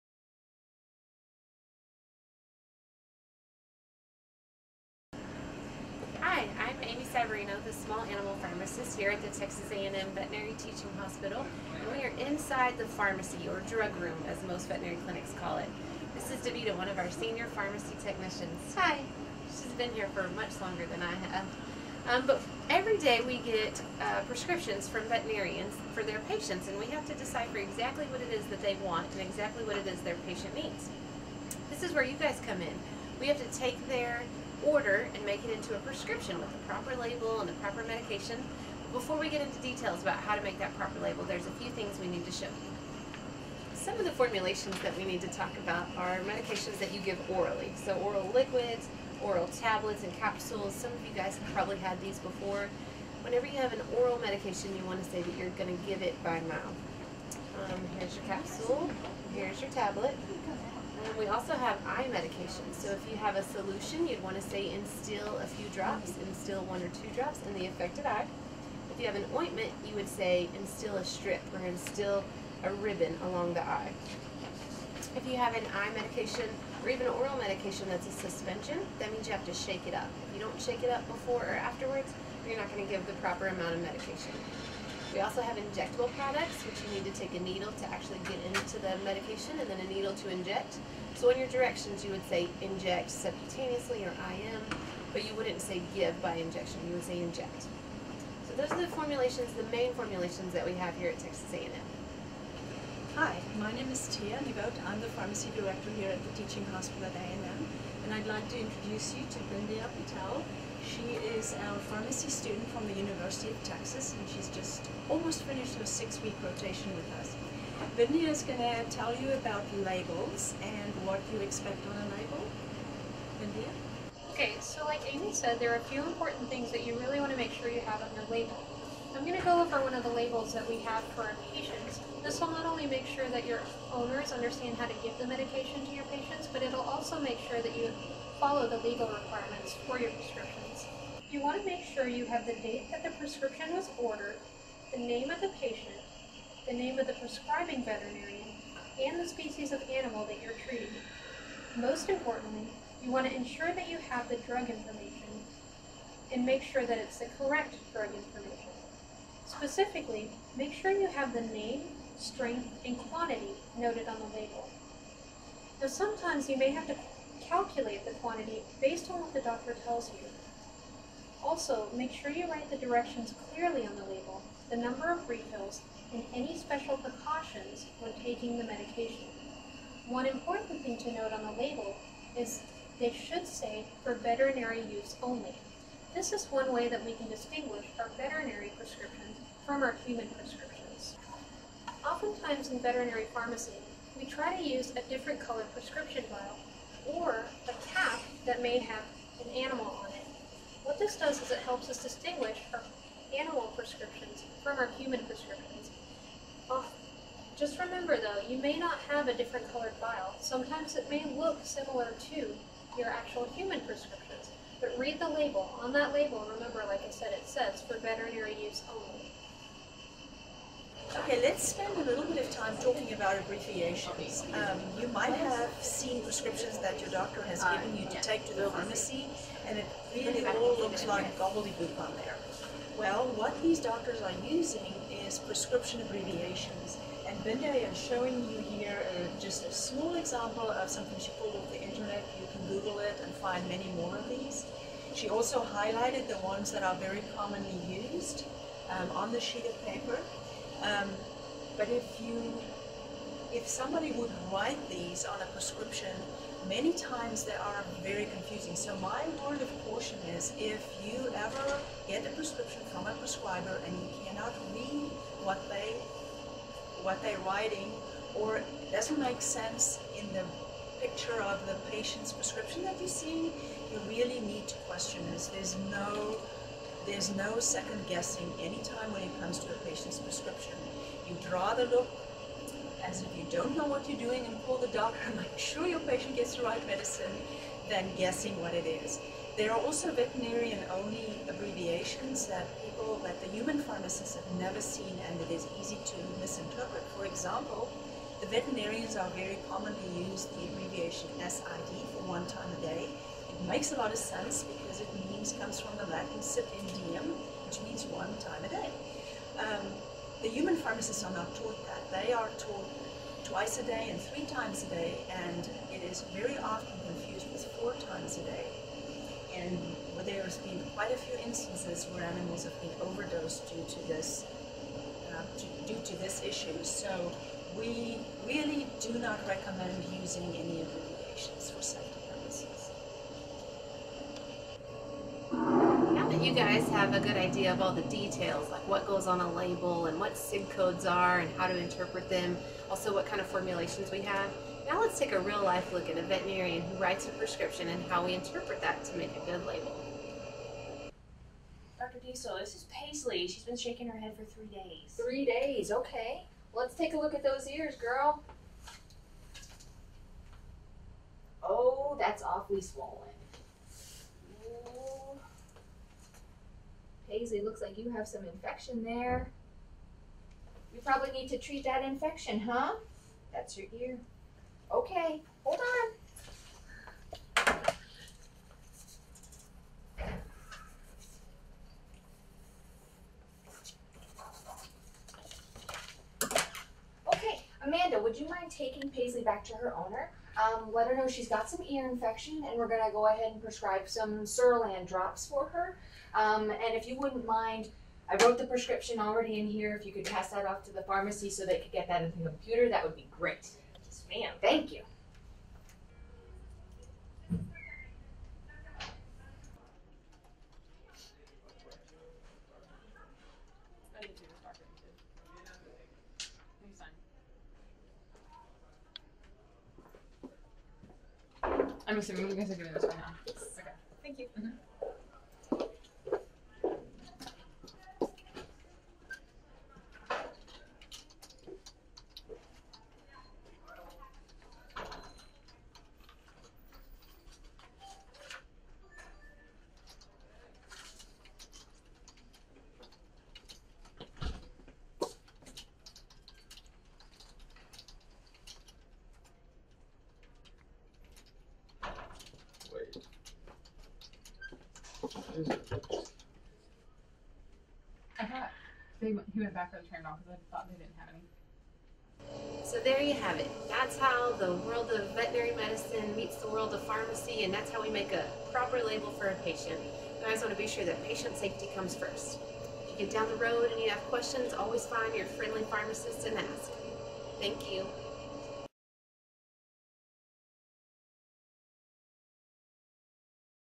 FFA- Filling A Prescription
Pharmacists at Texas A&M Teaching Hospital describe how to fill a prescription from veterinary orders.